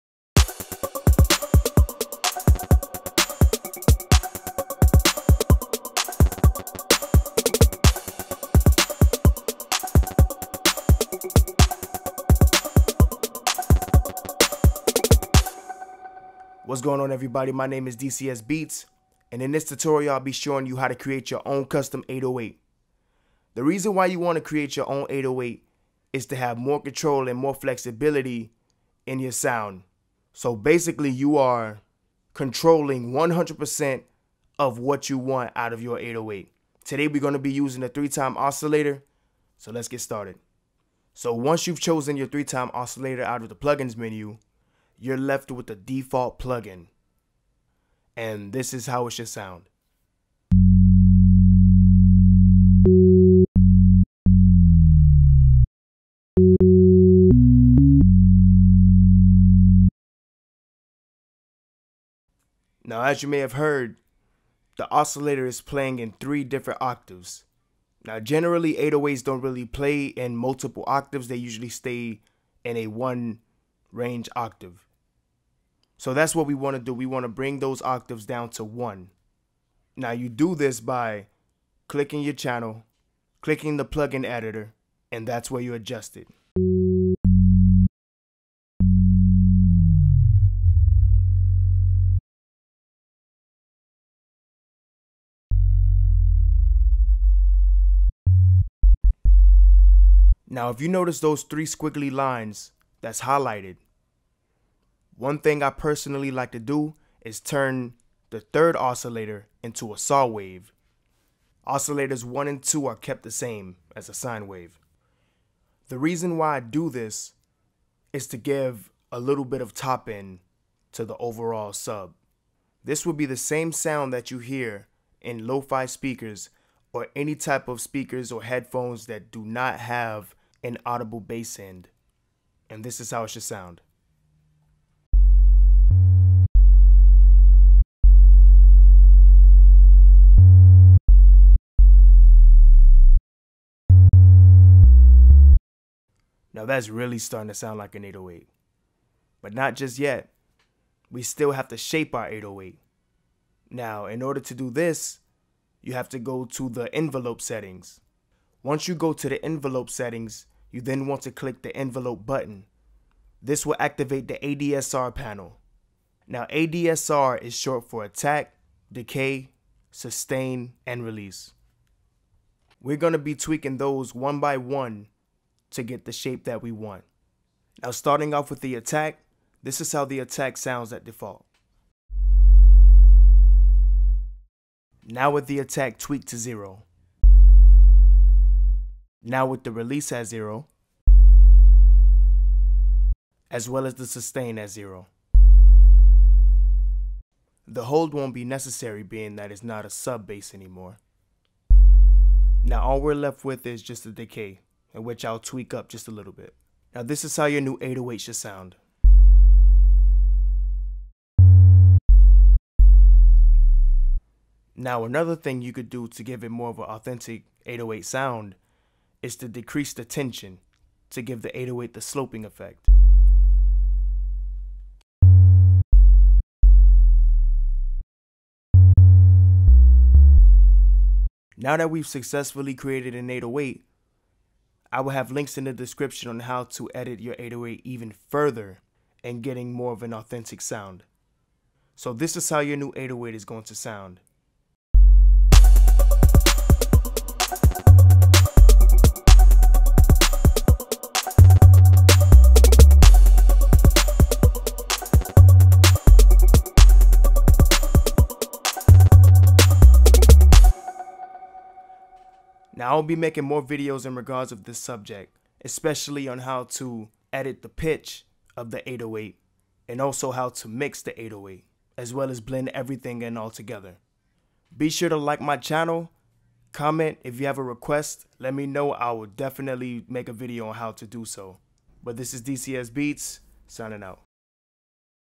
Fl Studio Tutorial- How to create your own 808 using 3x Osc
This is how to create your own 808 bass using the Fl Studio plugin.